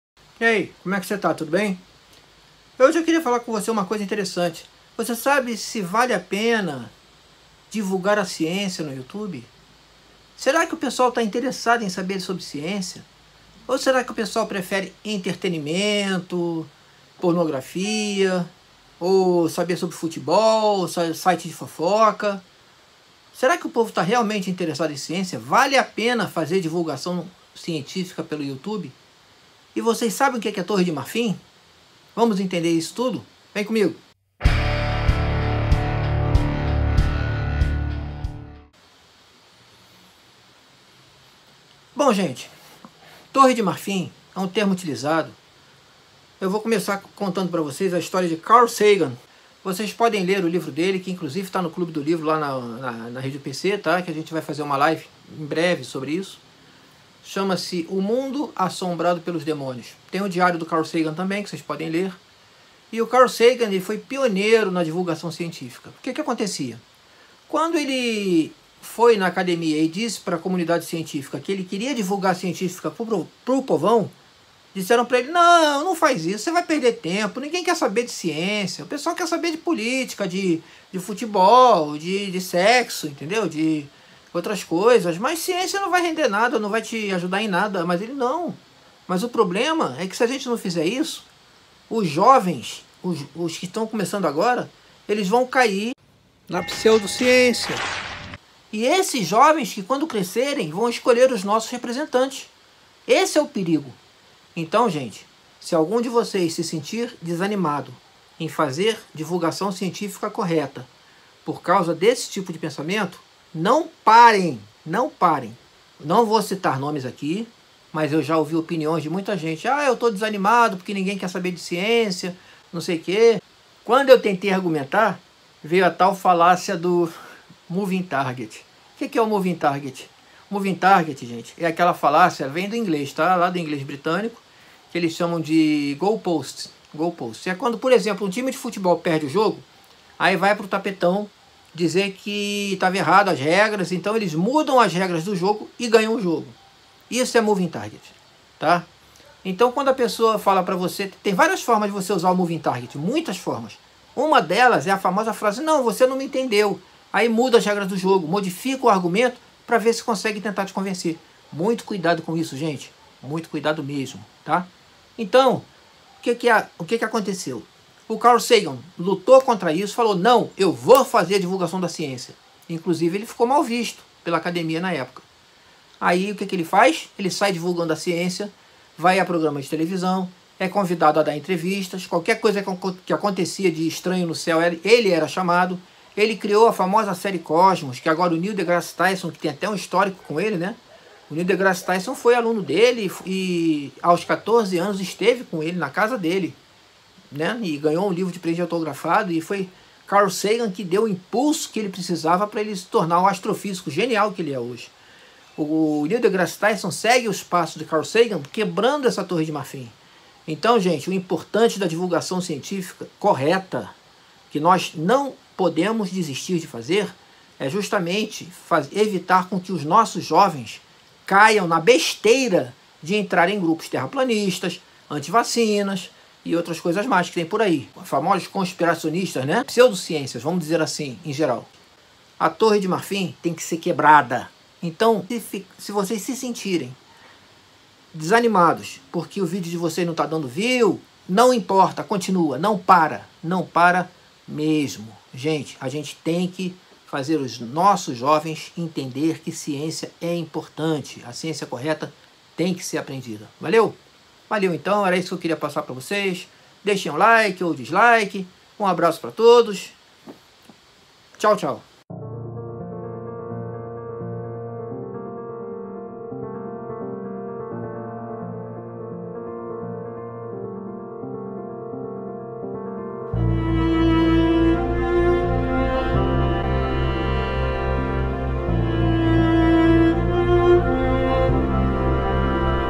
Vale a pena divulgar ciência no yutube? O que é a Torre de Marfim? Vem comigo!
FONTE:  Carl Sagan,  O mundo assombrado pelos demônios:
 (E toda a sua obra literária)

Apoiem-me, para que eu possa comprar um microfone decente, e poder continuar a fazer divulgação científica, amigos!  É só clicar aqui.  Qualquer quantia ajuda.  Desde já, obrigado!